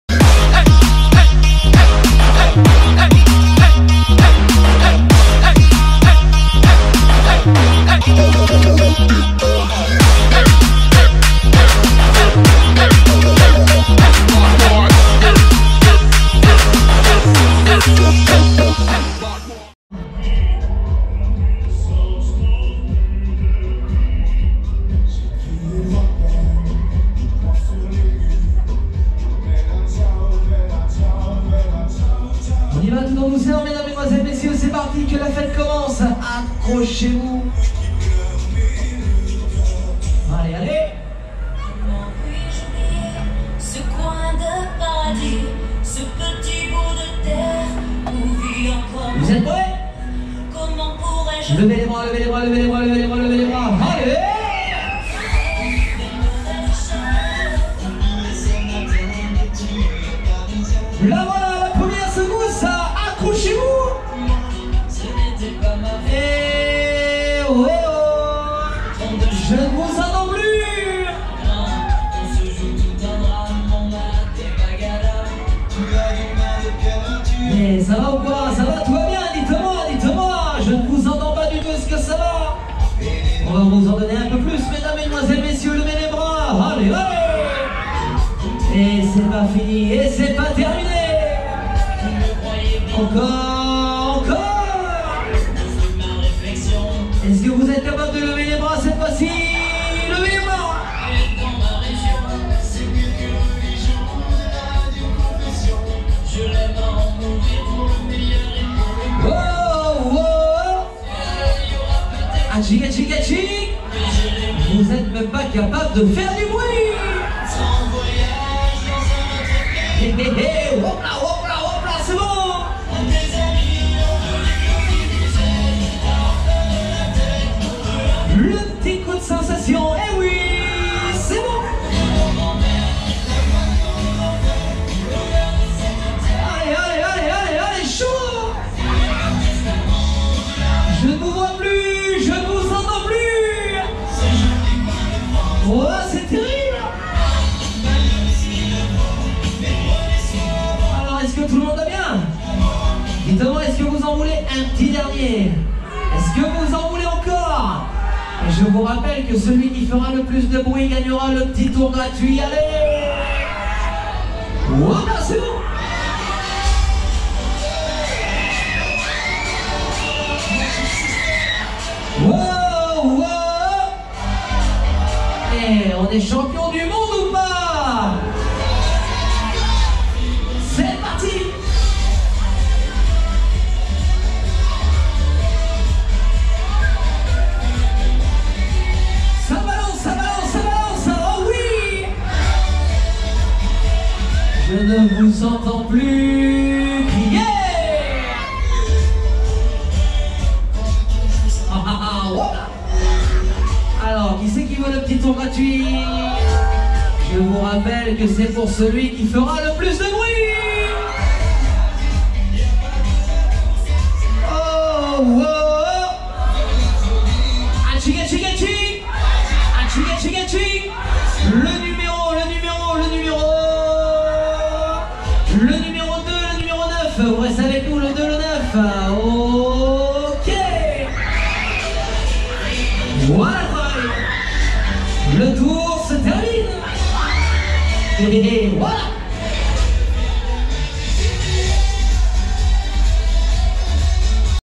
Planet Rock VUE DE LA CAISSE Fête foraine de St-Gengoux-Le-National 2019
Manège à sensations tenus par Mr Chassard.
Ce manège est un Surf TIVOLI.